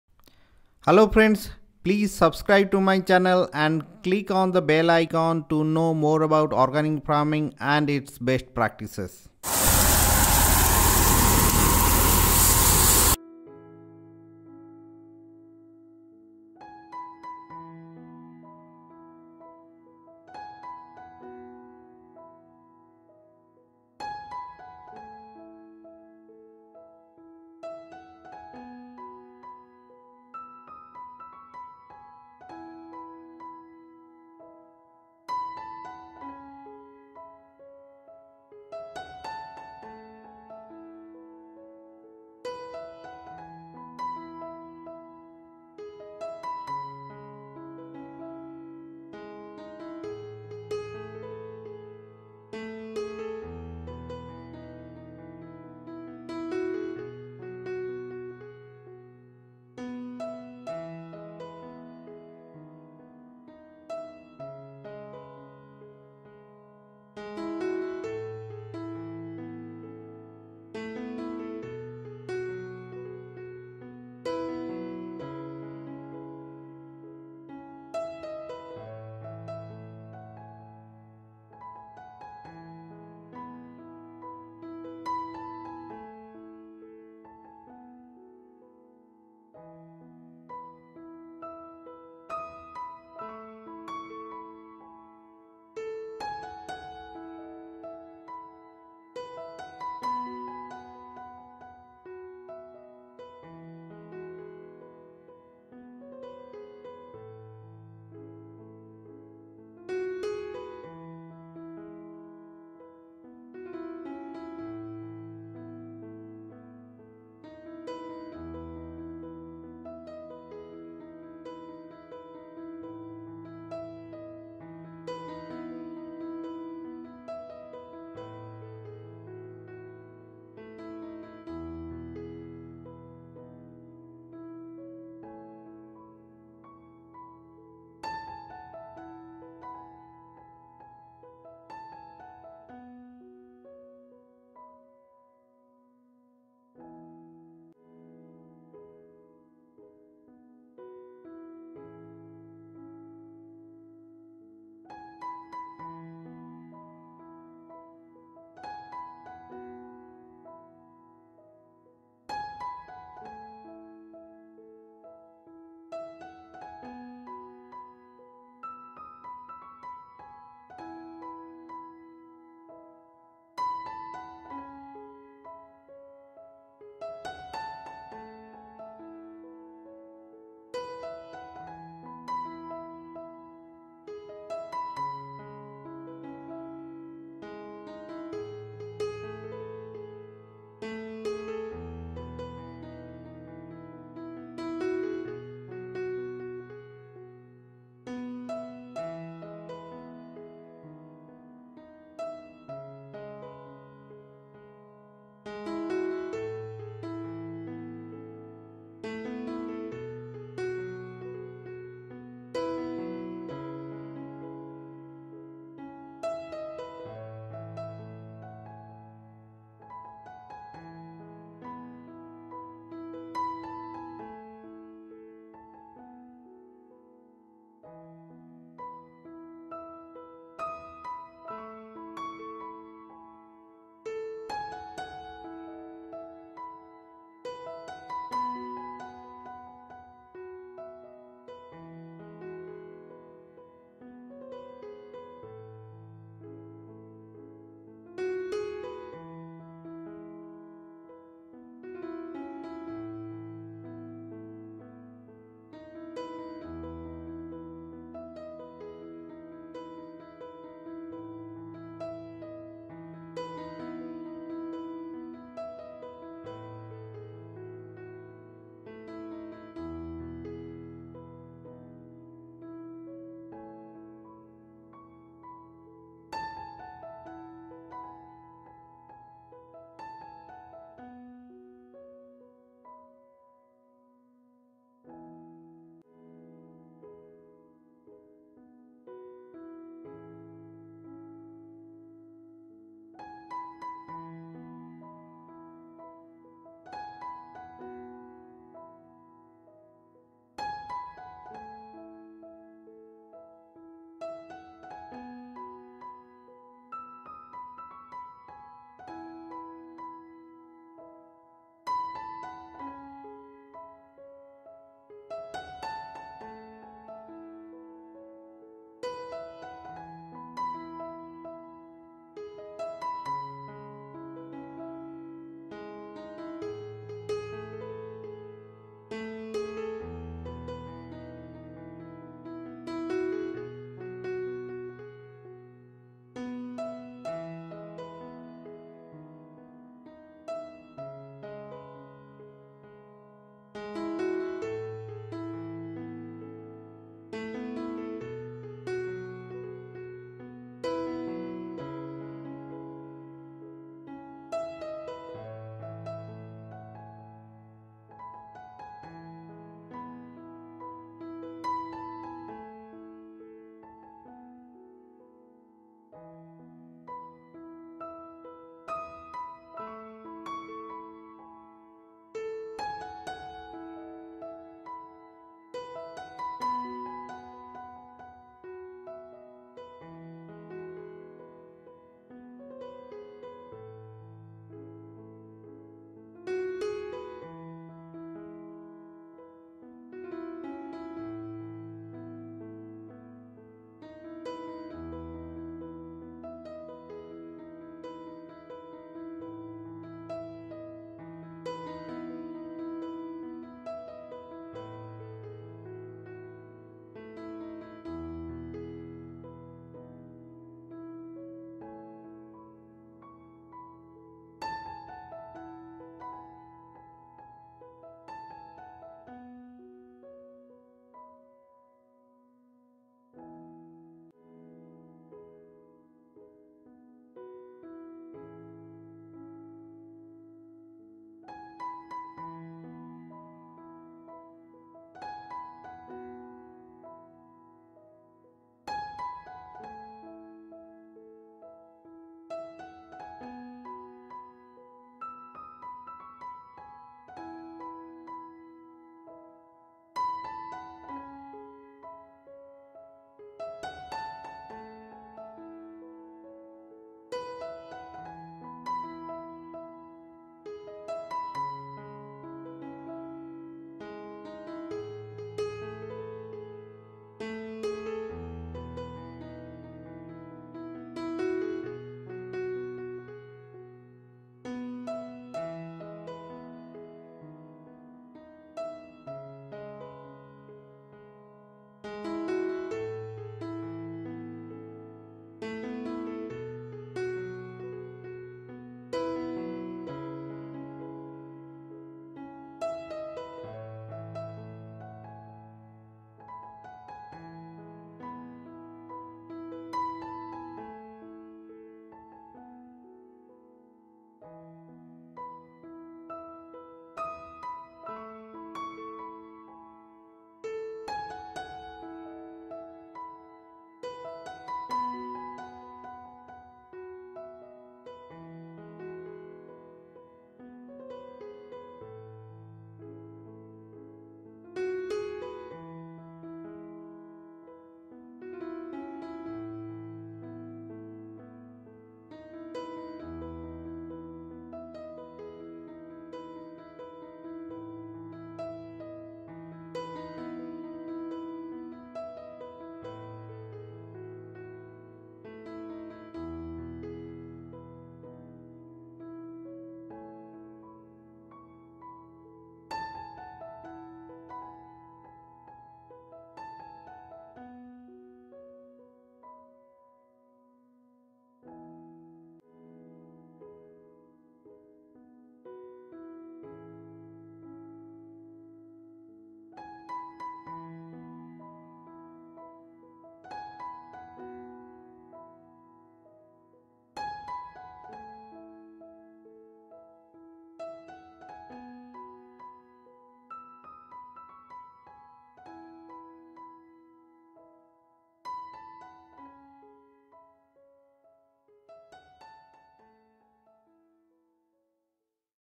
Rice Harvesting Machine in India
Rice Harvesting Machine makes farmers life easy. See this video, how easy it is to harvest rice.